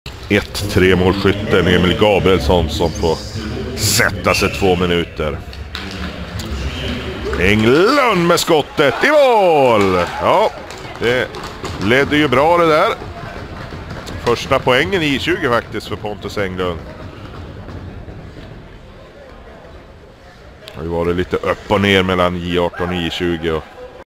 Timra's Englund scores first Swedish J20 goal -- 1/7/18
Timra IK J20 defenseman Pontus Englund, a prospect for the 2018 NHL Entry Draft, scored off a face-off at 4:33 of the second period of his team's 6-2 loss to Malmo in Swedish Under-20 action on Jan. 7, 2018. The goal was Englund's first at the SuperElit level.

NOTE: HockeyDraftCentral does not own any of this footage, which belongs entirely to its original rights-holder(s).